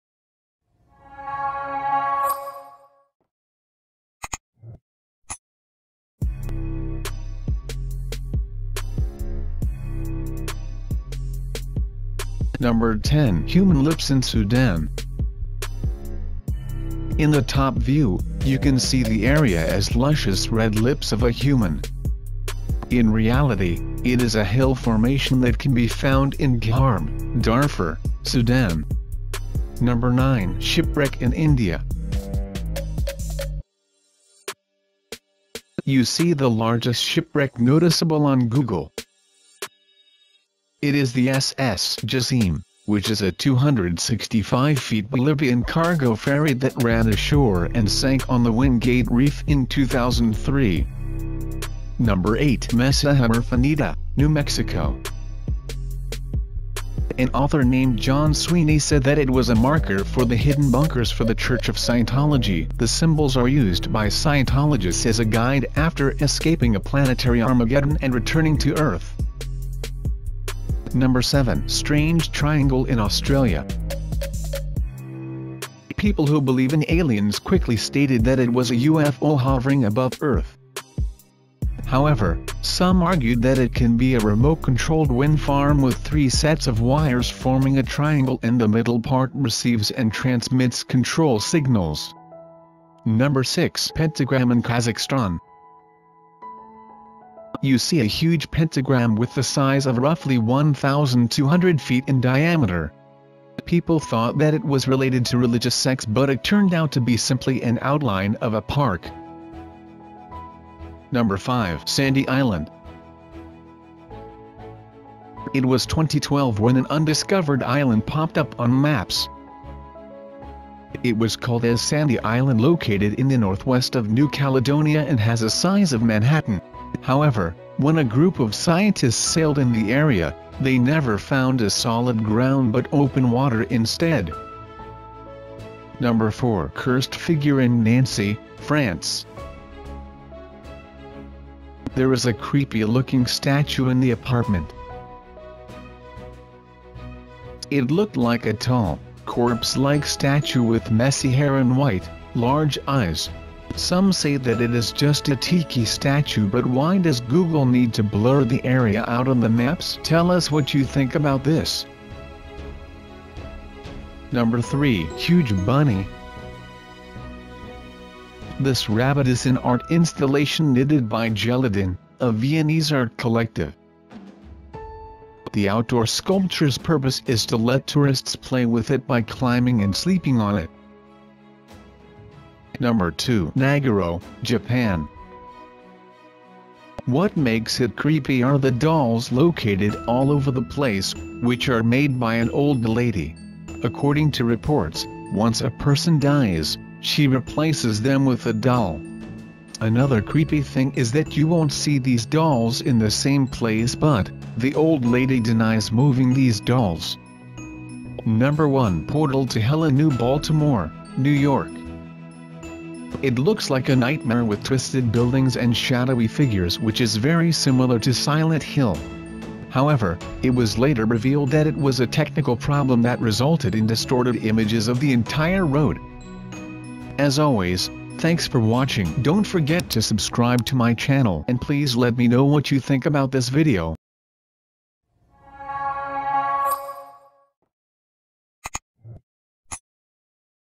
What Is This? Top 10 Images Caught On Google Earth That Nobody Can Explain
What Is This? 10 Images Caught On Google Earth That Nobody Can Explain

There are a lot of amazing places on the planet. Top 5s and 10s shows you a few unbelievable places that have been spotted using Google Earth.

Keywords;
viral, lists, facts, technology, food, health, top 10, top 5, life hacks,weird, strange